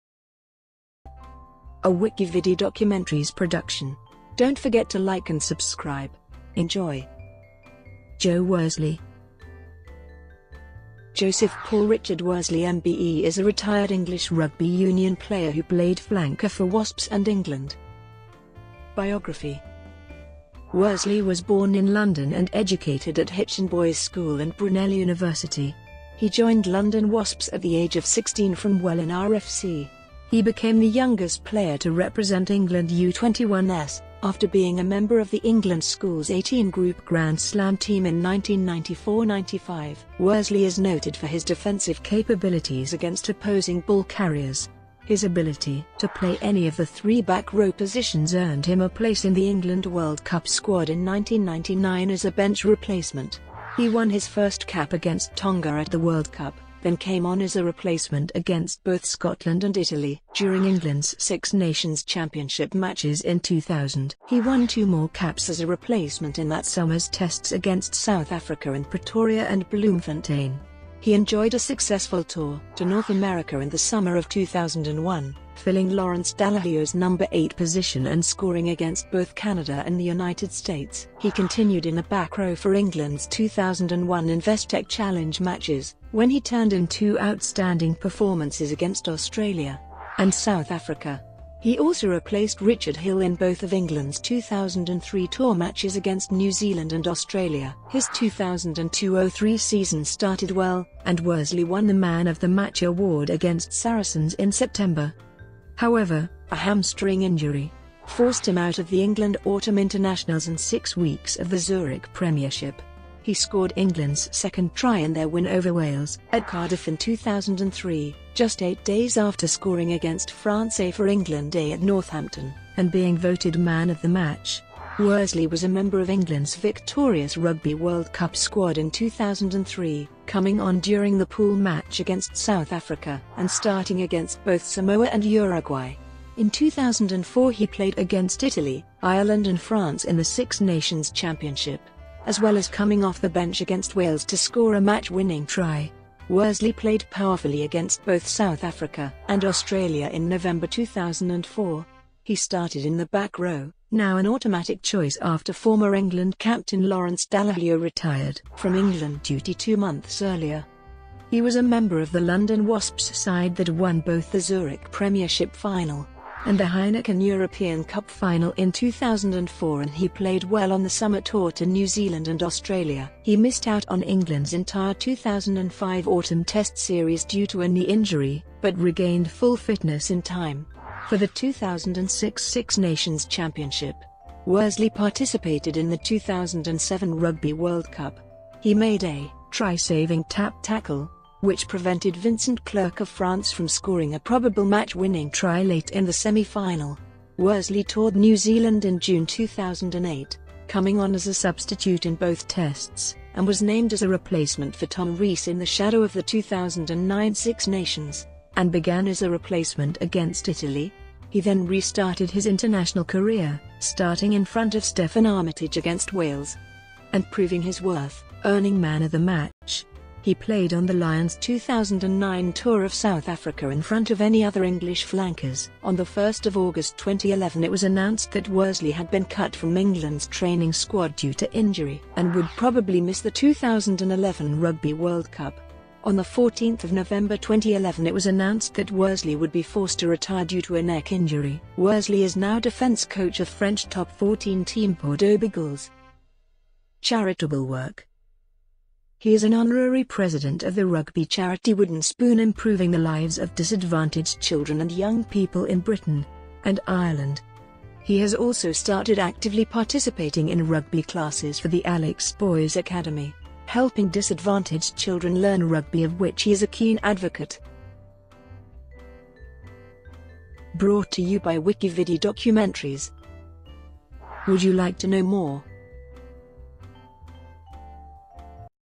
JOE WORSLEY - WikiVidi Documentary
Joseph Paul Richard Worsley MBE is a retired English rugby union player who played flanker for Wasps and England.

Shortcuts to chapters:
00:00:18 Biography

Copyright WikiVidi.